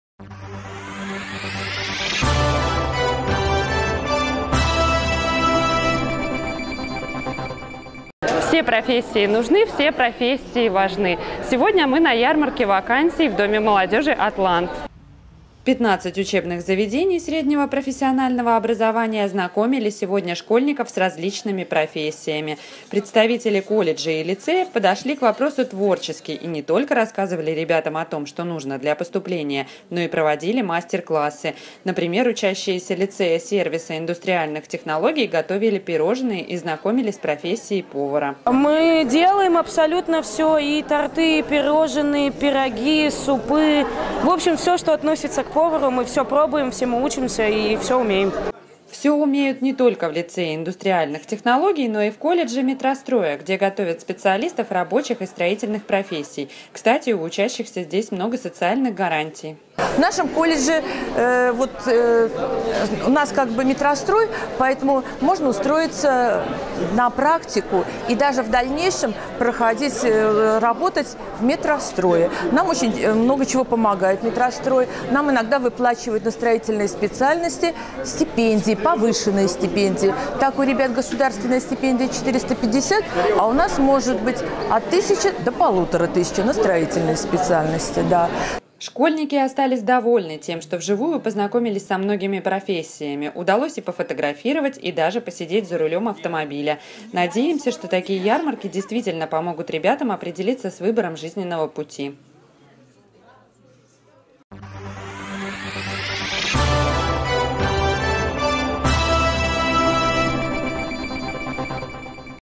Ярмарка Вакансий
Представители колледжей и лицеев знакомили  школьников с различными профессиями.